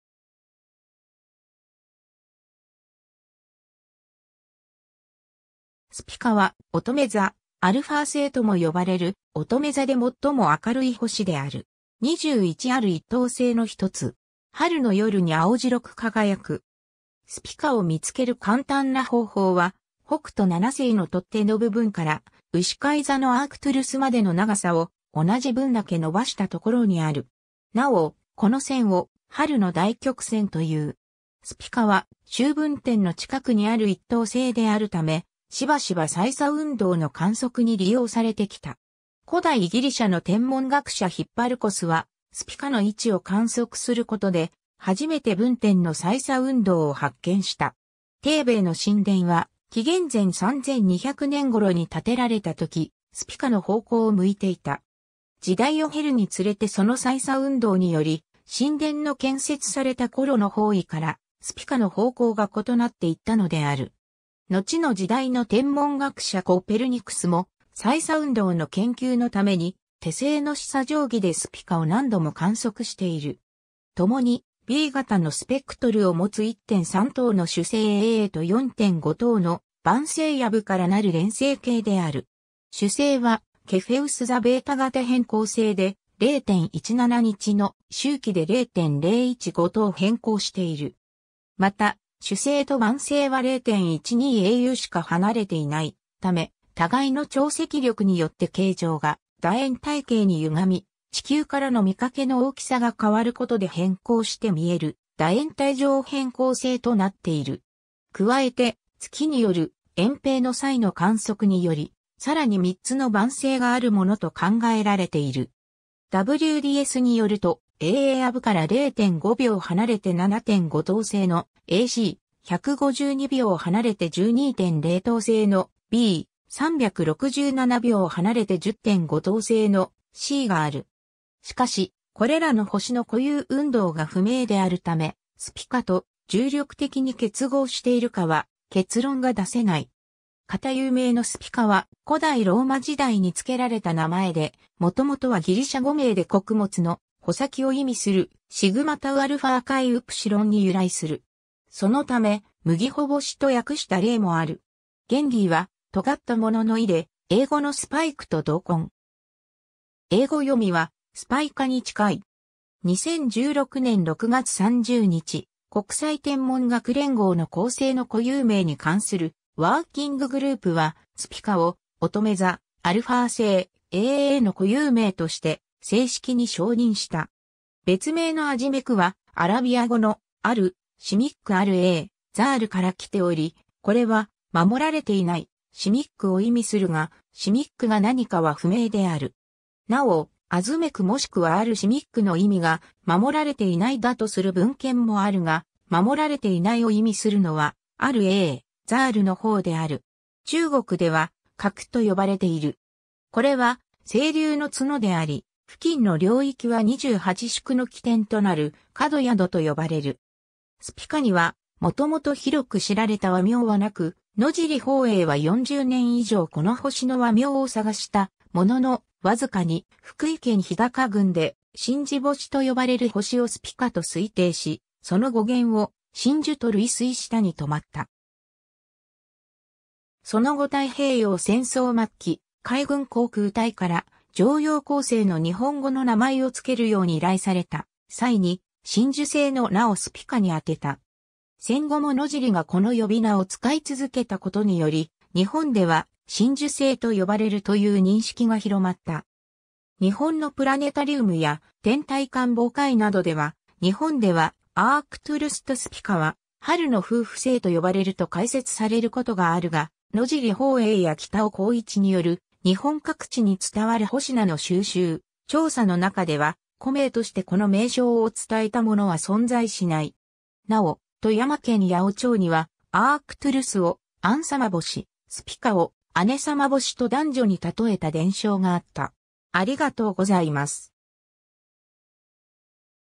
スピカ
スピカ (Spica, α Virginis: α Vir) はおとめ座α星（おとめざアルファせい）とも呼ばれる、おとめ座で最も明るい星（恒星）である。
21ある1等星（全天21の一等星）の1つ。
春の夜に青白く輝く。
 スピカを見つける簡単な方法は、北斗七星の取っ手の部分からうしかい座のアークトゥルスまでの長さを同じ分だけ伸ばした所にある。
なお、この線を春の大曲線という。
 スピカは秋分点の近くにある1等星であるため、しばしば歳差運動の観測に利用されてきた。
古代ギリシャの天文学者ヒッパルコスはスピカの位置を観測することで初めて分点の歳差運動を発見した。
テーベの神殿は紀元前3200年頃に建てられた時、スピカの方向を向いていた。
時代を経るにつれてその歳差運動により、神殿の建設された頃の方位からスピカの方向が異なっていったのである。
のちの時代の天文学者コペルニクスも、歳差運動の研究のために、手製の視差定規でスピカを何度も観測している。
 共にB型のスペクトルを持つ1.3等の主星Aaと4.5等の伴星Abからなる連星系である。
主星はケフェウス座β型変光星で、0.17日の周期で0.015等変光している。
また、主星と伴星は0.12auしか離れていないため、互いの潮汐力によって形状が楕円体型に歪み、地球からの見かけの大きさが変わることで変光して見える楕円体状変光星となっている。
 加えて、月による掩蔽の際の観測により、さらに3つの伴星があるものと考えられている。
WDSによると、Aa+Abから0.5秒離れて7.5等星のAc、152秒離れて12.0等星のB、367秒離れて10.5等星のCがある（1秒は77天文単位に相当）。
しかし、これらの星の固有運動が不明であるため、スピカと重力的に結合しているかは結論が出せない。
 固有名のスピカ (Spica) は、古代ローマ時代に付けられた名前で、もともとはギリシャ語名で穀物の「穂先」を意味する Σταχυς に由来する。
そのため「麦穂星」と訳した例もある。
原義は「尖ったもの」の意で、英語のスパイク（Spike）と同根。
英語読みはスパイカに近い。
2016年6月30日、国際天文学連合の恒星の固有名に関するワーキンググループは、Spica をおとめ座α星 Aa の固有名として正式に承認した。
 別名のアジメク (Azimech) は、アラビア語の「السماك الأعزل al-simāk al-ʼaʽzal」から来ており、これは「守られていないsimāk」を意味するが、simāk が何かは不明である。
なお、Azimechもしくは al-simāk の意味が「守られていない」だとする文献もあるが、「守られていない」を意味するのは al-ʼaʽzal の方である。
 中国では「角」と呼ばれている。
これは青龍のツノであり、付近の領域は二十八宿の起点となる角宿と呼ばれる。
 スピカには元々広く知られた和名はなく、野尻抱影は40年以上この星の和名を探したものの、わずかに福井県日高郡で「しんじぼし」と呼ばれる星をスピカと推定し、その語源を「真珠」と類推したに留まった。
その後太平洋戦争末期、海軍航空隊から常用恒星の日本語の名前を付けるように依頼された際に「真珠星」の名をスピカに充てた。
戦後も野尻がこの呼び名を使い続けたことにより、「日本では「真珠星」と呼ばれる」という認識が広まった。
 日本のプラネタリウムや天体観望会などでは、「日本では、アークトゥルスとスピカは「春の夫婦星」と呼ばれる」と解説されることがあるが、野尻抱影や北尾浩一による日本各地に伝わる星名の収集・調査の中では、古名としてこの名称を伝えたものは存在しない。
なお、富山県八尾町（現・富山市）には、アークトゥルスを「アンサマボシ（兄）」、スピカを「アネサマボシ（姉）」と男女にたとえた伝承があった。